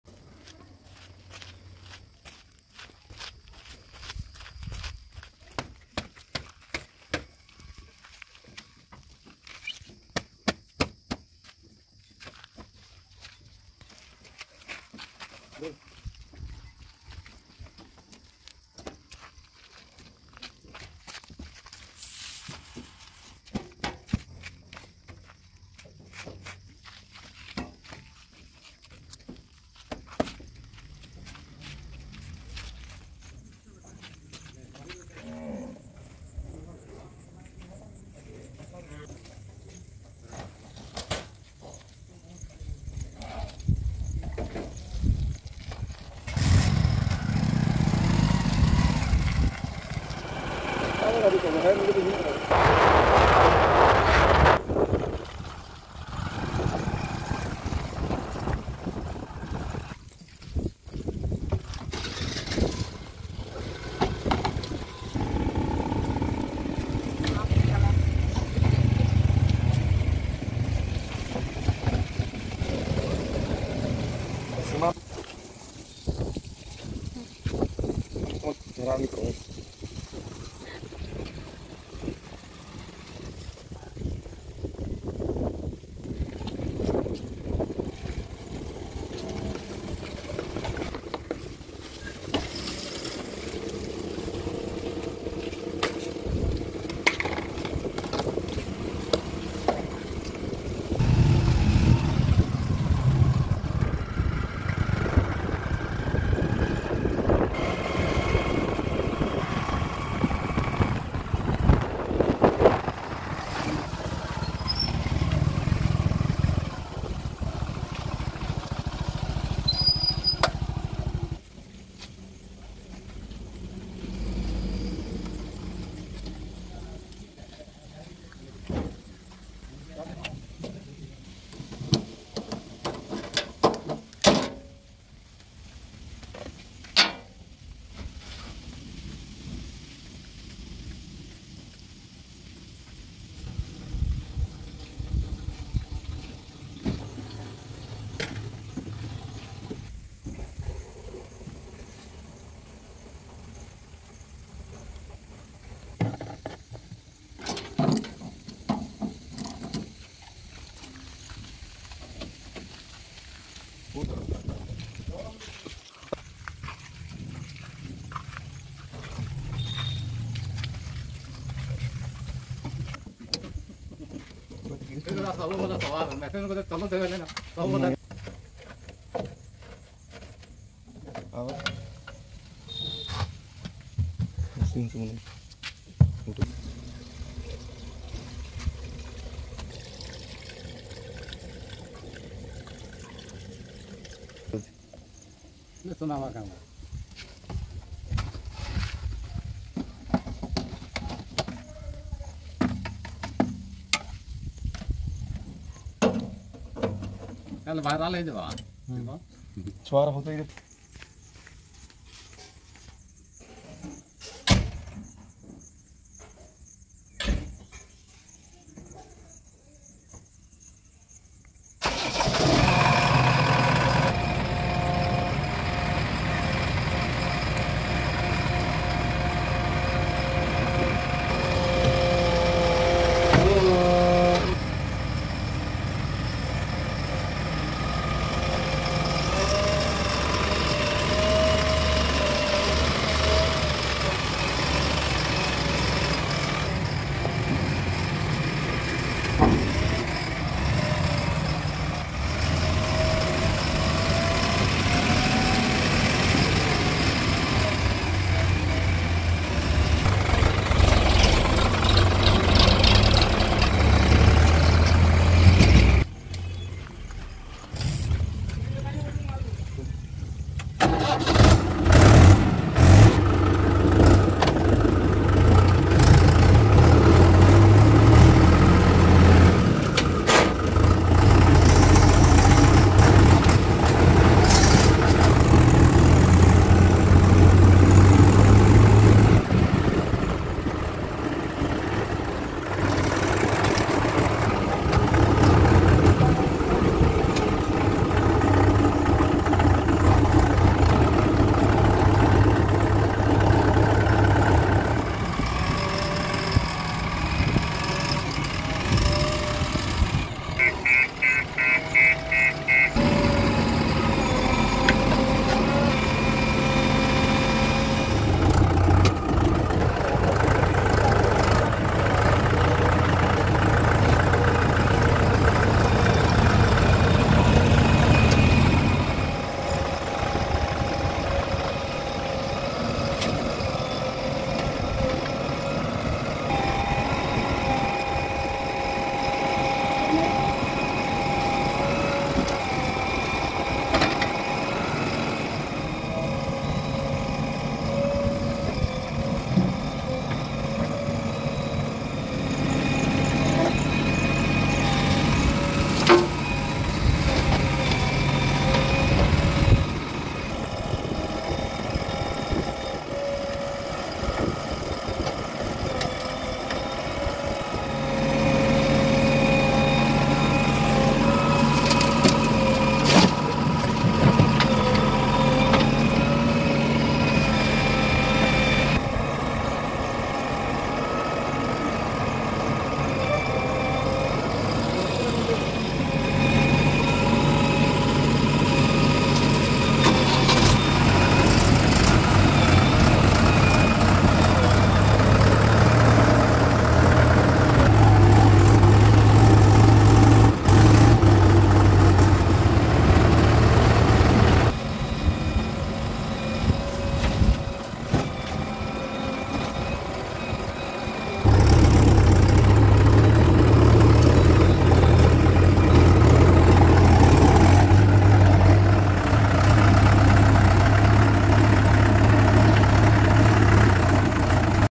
Jcb 3dx Backhoe Loader Loading Red Mud In Mahindra and Swaraj Tractor | Jcb and Tractor Cartoon
About Video: In this video a jcb 3dx backhoe loader machine loading mud in Mahindra and Swaraj tractors 🚜 😀

By this video you can watch the performance of jcb machine and both Mahindra and Swaraj tractors power 💪 🚜